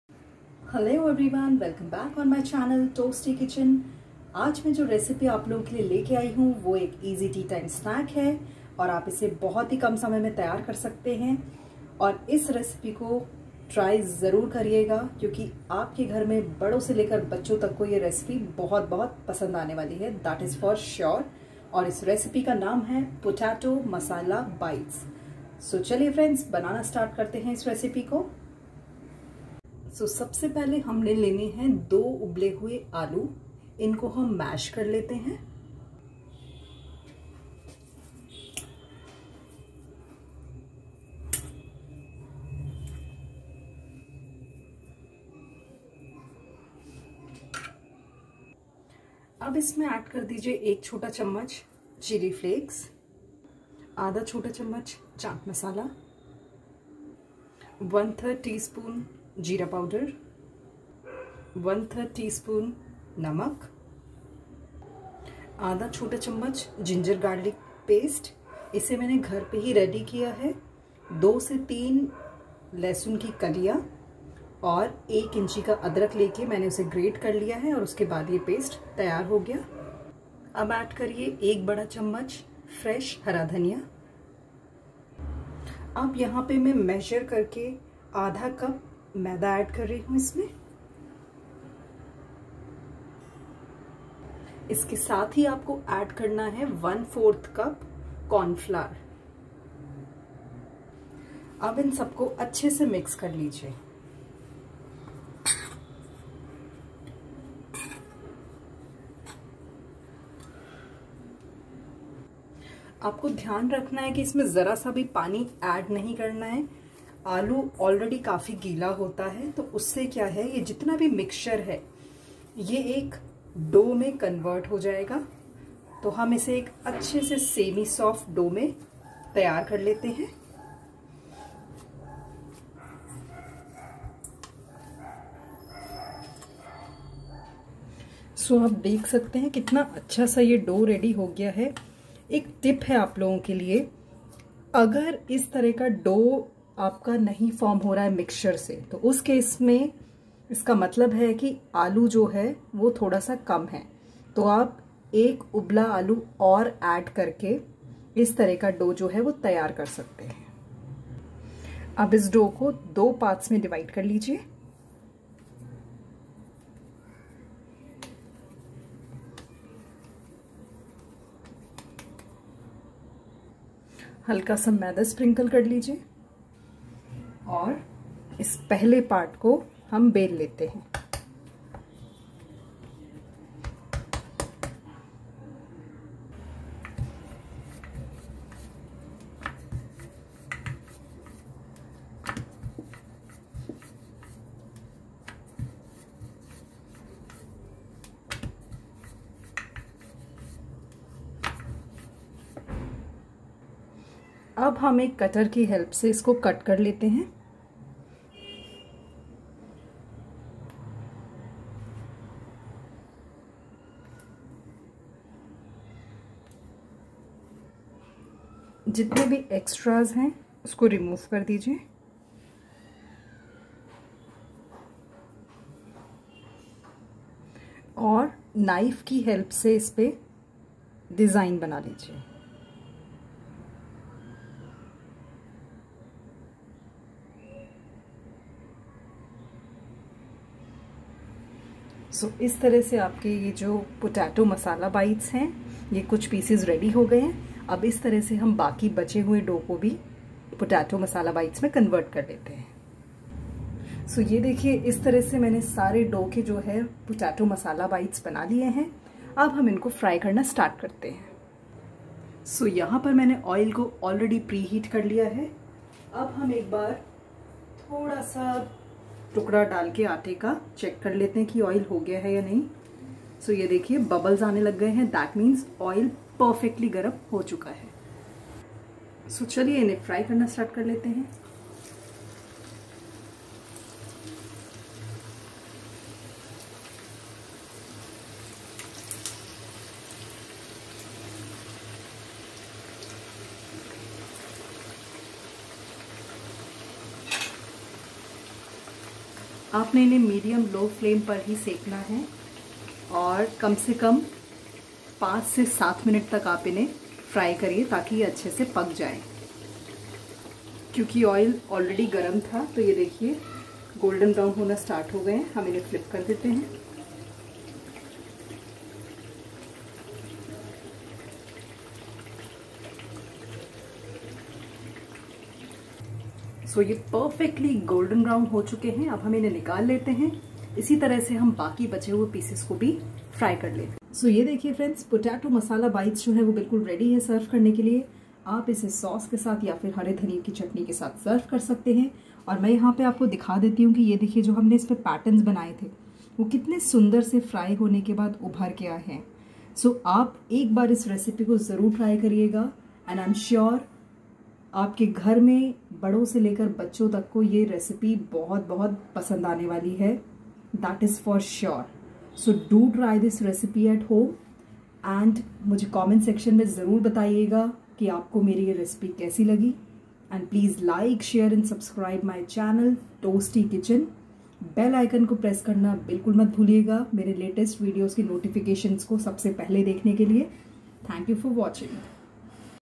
Potato Masala Bites|Potato Masala Parantha Bites|Spicy Potato Snacks|Toasty Kitchen
Sharing before you all an amazing recipe of Potato masal bites...Link is given below:-

INGREDIENTS & PROCESS:
2 Boiled Mashed Potatoes
1 Tsp Chilli Flakes
½ Tsp Chat Masala
½ Tsp Cumin Powder
1/3 Tsp Salt
½ Tspn Garlic Ginger Paste
1 Tbsp Coriander Leaves
½ Cup Flour
¼ Cup Corn Flour
Mix Well
Take Some Dough
Roll
Cut With a Steel Bowl
Make Pattern
Fry in Medium Heated Oil
Fry For 5-7 Min.